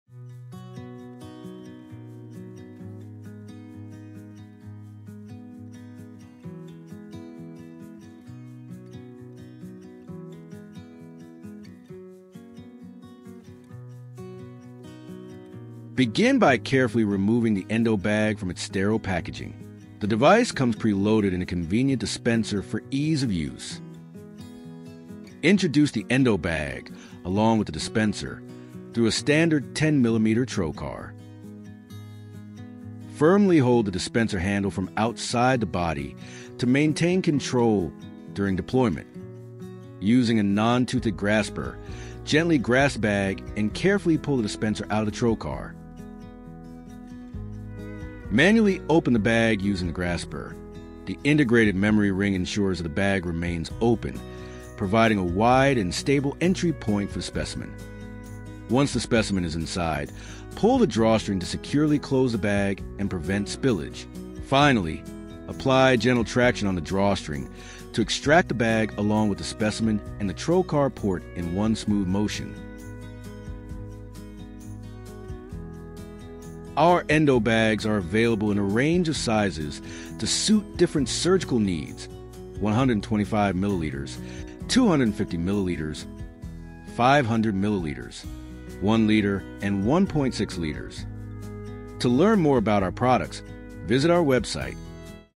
How to use Endobag? (Disposable Specimen Retrieval Bag)
In this informative and easy-to-follow video, we walk you through the step-by-step process of using the Morrisons Endobag. From unpacking the Endobag to safely removing the specimen, this video provides a comprehensive guide for medical professionals. Whether you’re a seasoned surgeon or a medical student, this video serves as a valuable resource for understanding and mastering the use of the Morrisons Endobag in surgical procedures. Watch now to enhance your surgical skills and knowledge!